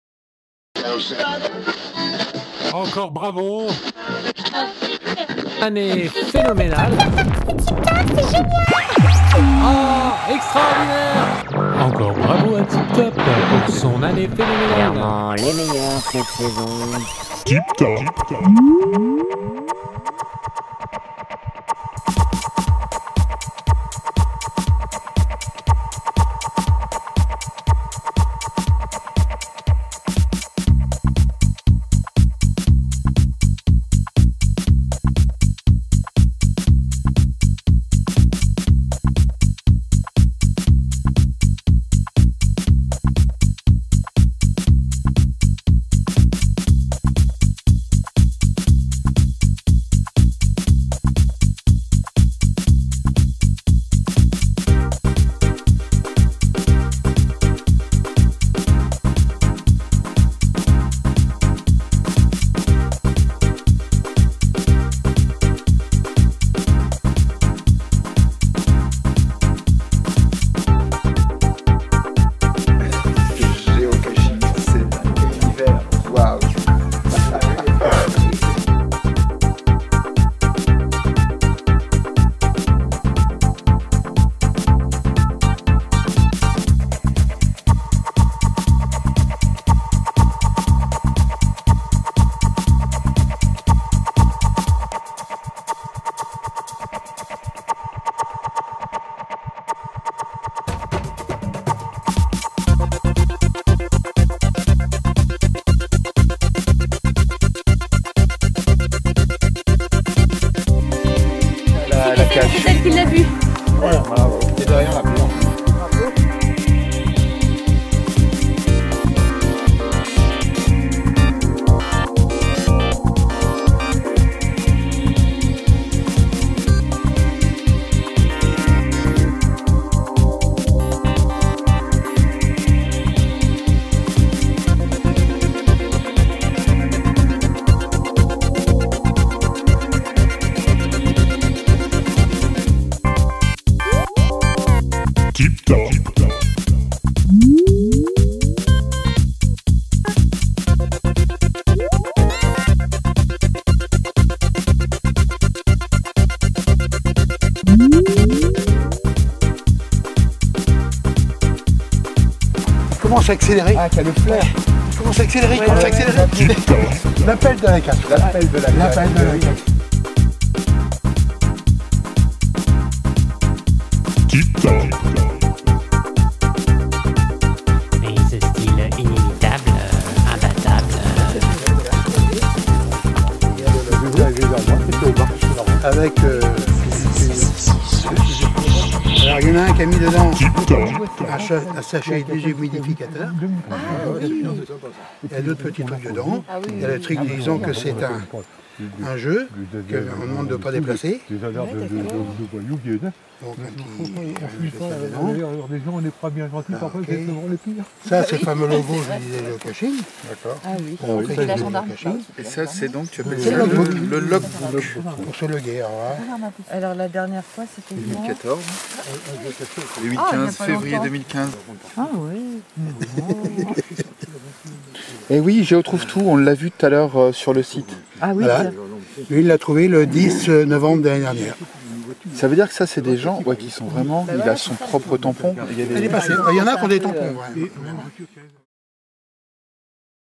TIPTOP 2015 geocaching
Séance découverte du géocaching organisée par l'association TIPTOP à la mairie de Trévron, avec Didier Voisin.